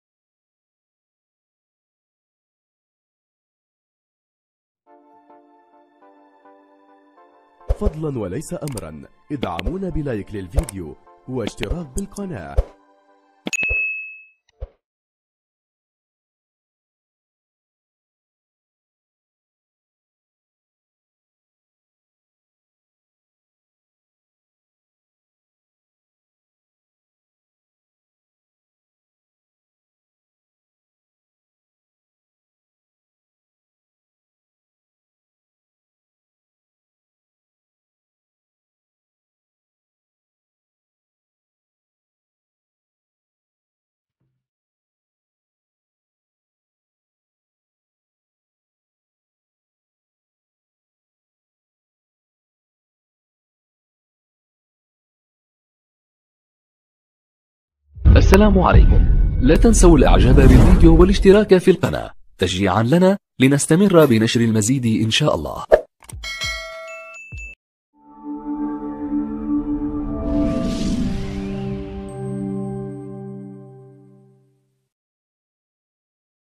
جدول تسليم المفاتيح QR19 موقع 2110/5000 مسكن سيدي عبد الله مصحوبين بالملف 2نسخ ب/ت/و +2نسخ العقد+2AF
La suite de Planning a partir de Mardi nchalah Mabrouk alikom hamdoulah c bon
Dossier :
2 affectation
2 copie l'engagement
2 copie pièce d'identité